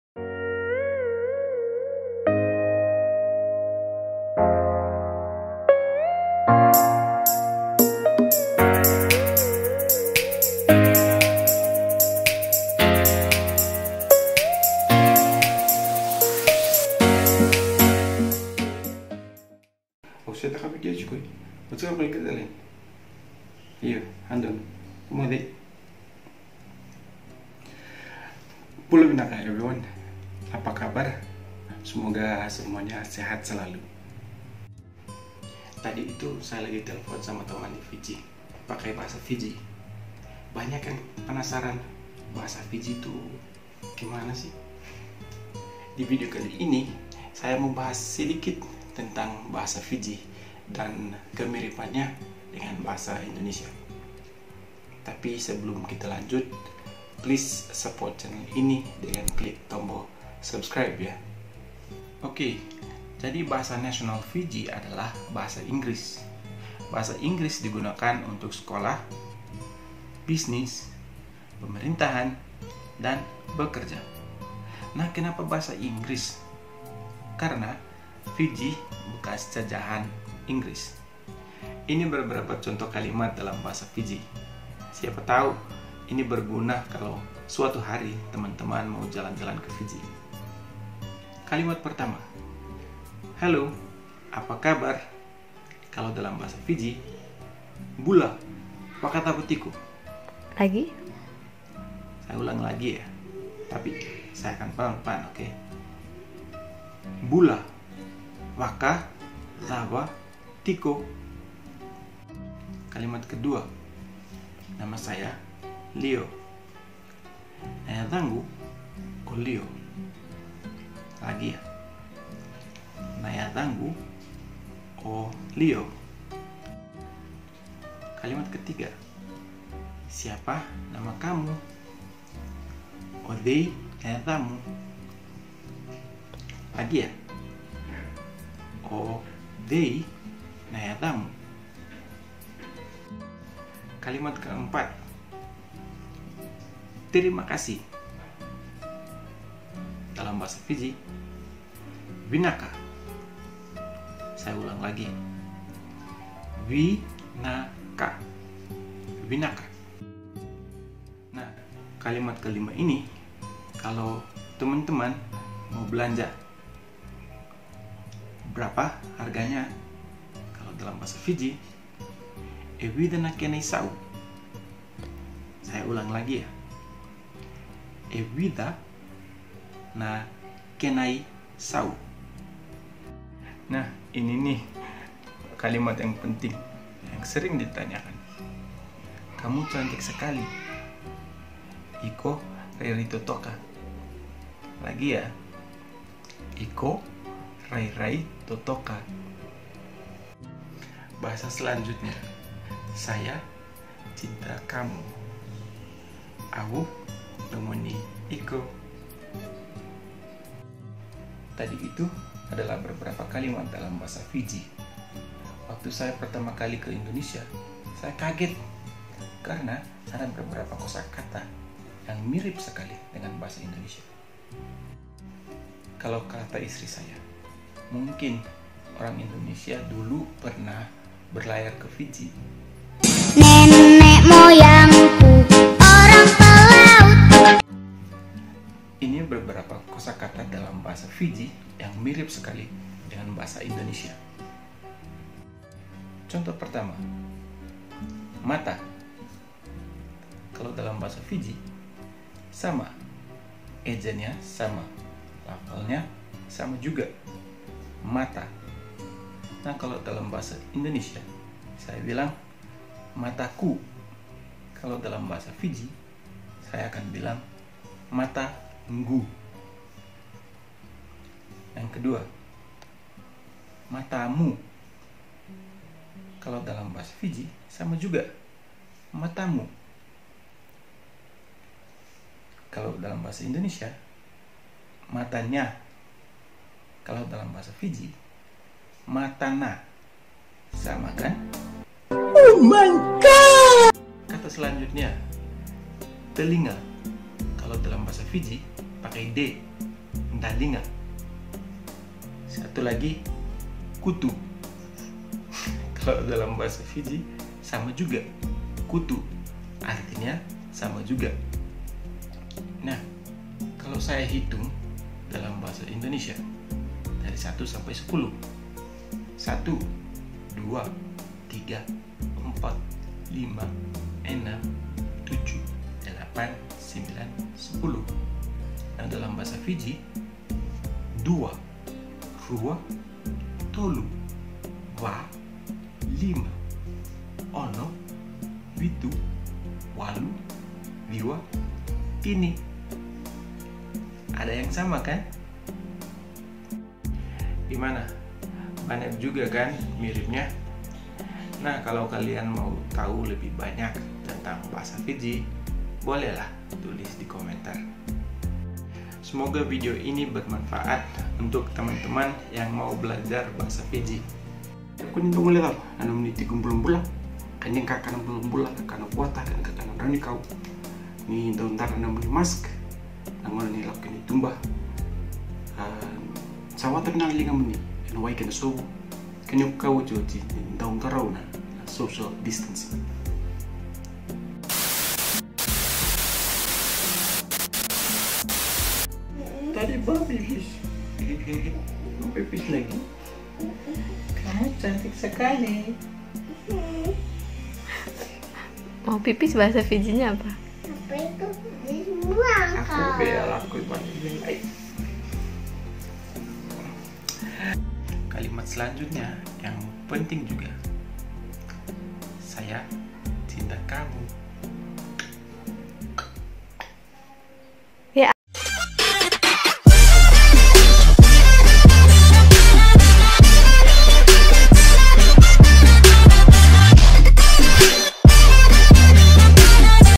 Belajar Bahasa Fiji, Banyak kosakatanya mirip Bahasa Indonesia Lho! (Learning Fiji Language)
Bula Vinaka! Selamat Datang! and Welcome to LEO FIJI YouTube Channel.

Let's learn the Fijian language.